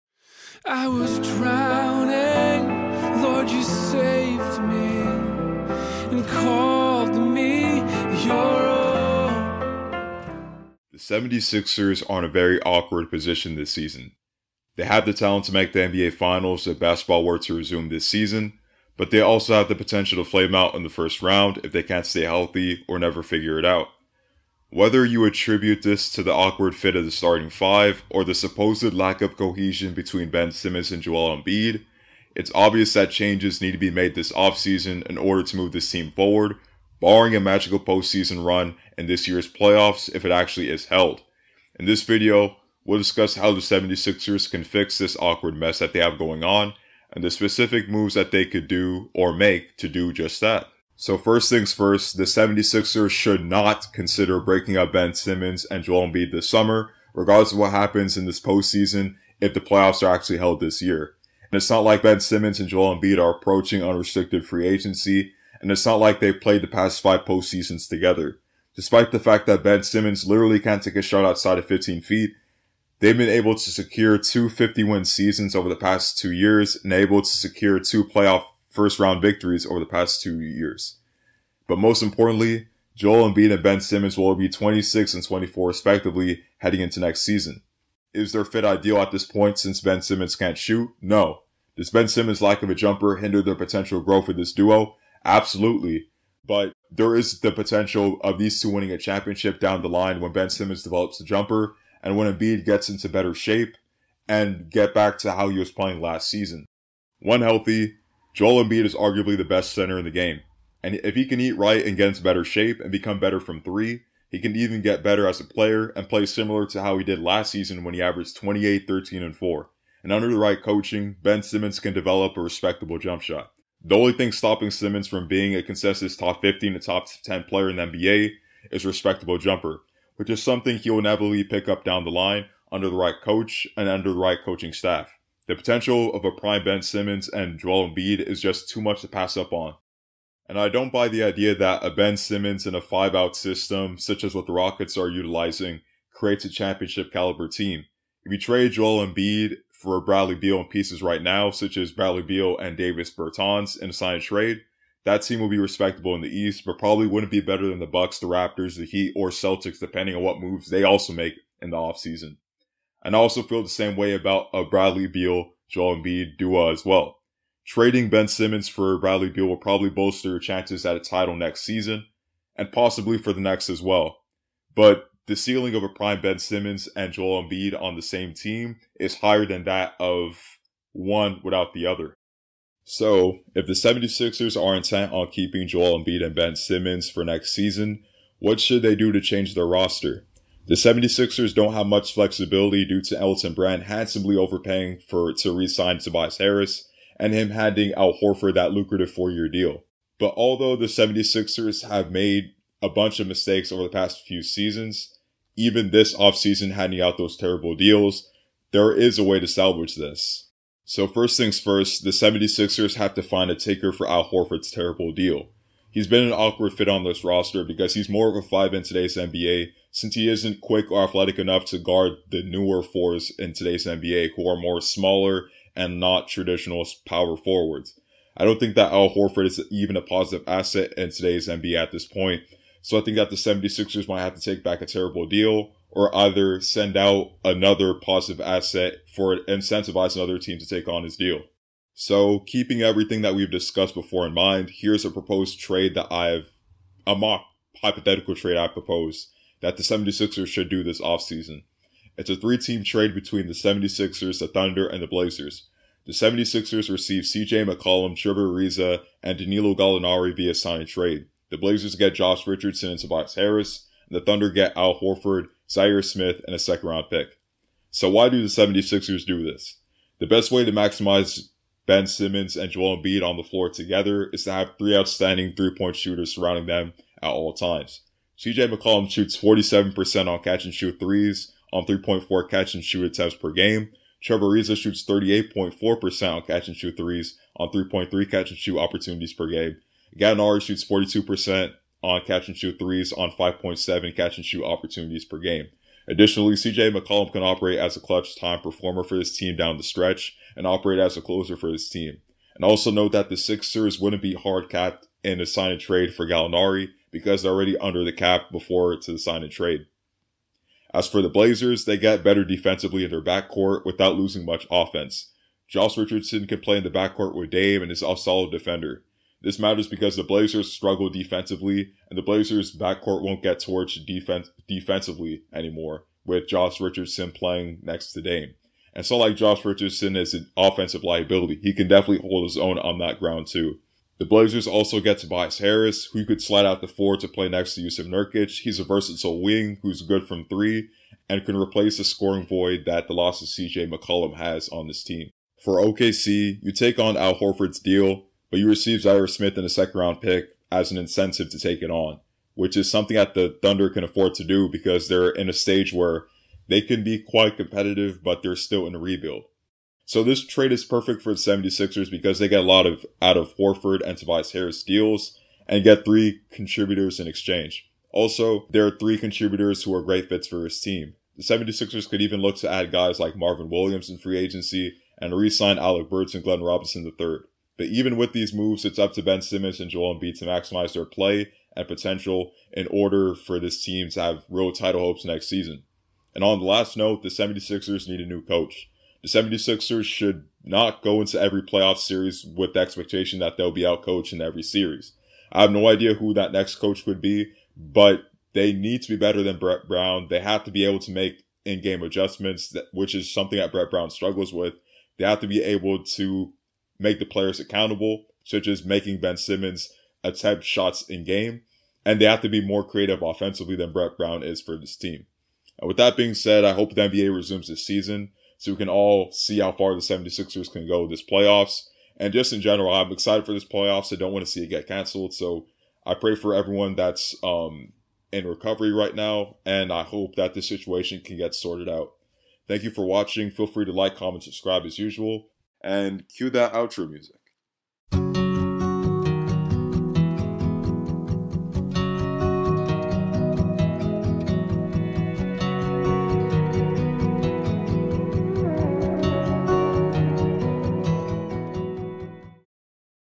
How To Fix the Philadelphia 76ers
Note:
I acknowledge that the proposed trade isn't extremely realistic, but the premise as to what the 76ers should do this offseason should be the same. This was recorded 1000 years ago, so the Blazers presumptively aren't trading CJ anymore, and OKC aren't taking on Horford's deal for the compensation listed in this video.

The 76ers are in a very awkward position this season. They have the talent to make the NBA Finals this if basketball were to resume, but they also have the potential to flame out in the first round if they can’t stay healthy or never figure it out. Whether you attribute this to the awkward fit of the starting 5, or the supposed lack of cohesion between Ben and Embiid, it’s obvious that changes need to be made this offseason in order to move the team forward barring a magical postseason run. In this video, we’ll be discuss how the 76ers can fix this awkward mess they have going on and specific moves they could make to do just that.

Thumbnail art: rattraptees (via Redbubble)